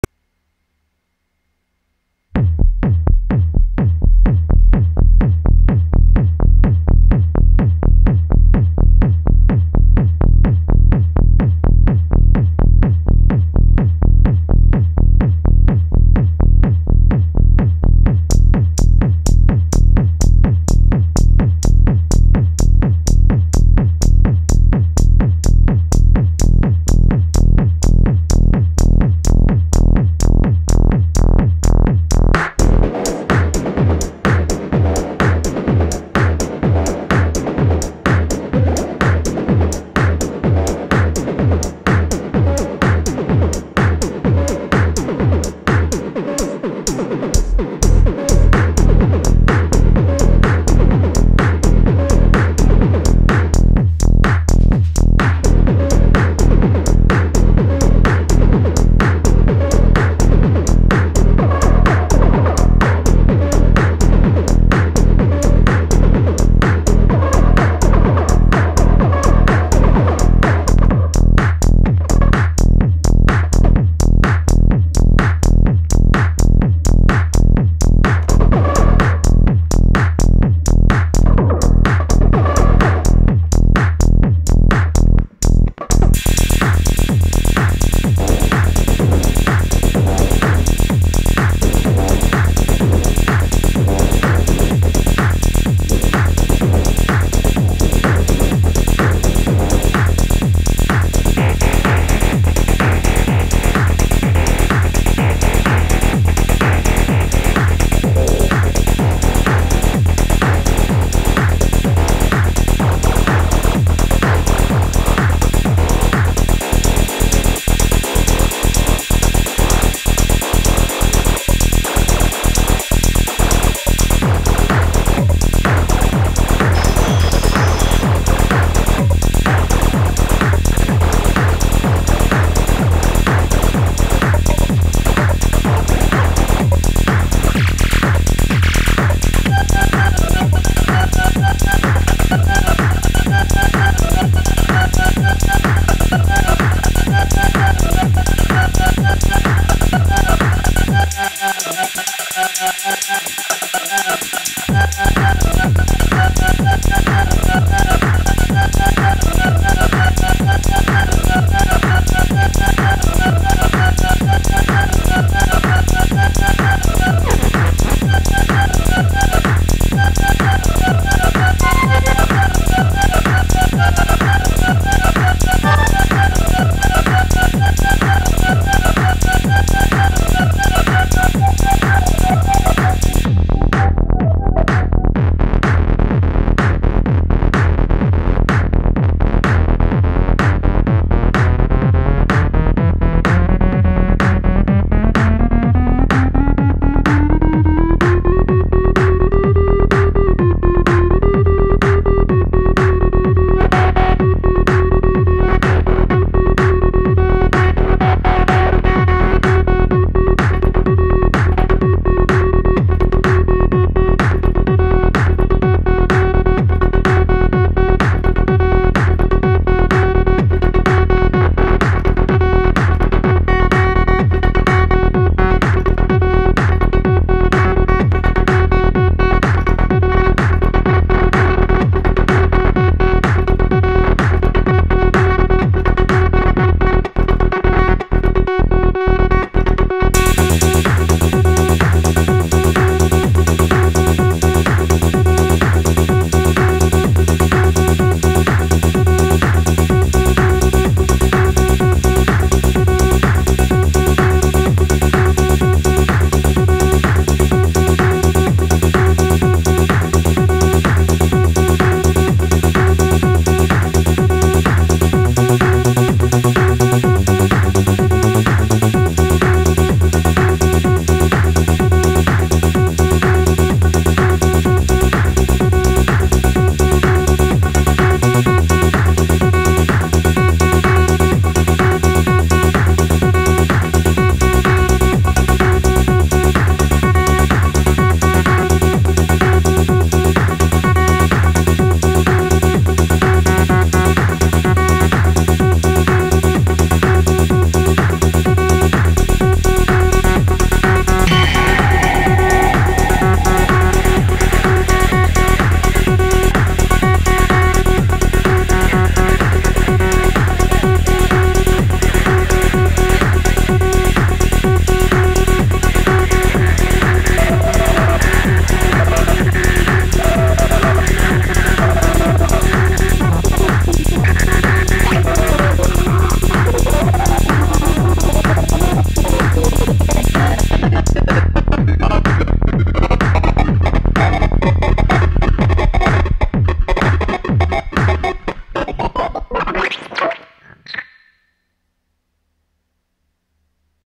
Techno TRANCE live Jam // Prophet 6 into Bastl Thyme 🤯
Berlin Techno live performance with Prophet 6, Bastl Thyme, Volca Bass, Analog Rytm and Arturia beatstep pro.

*Added some limiting/compression to the recorded performance.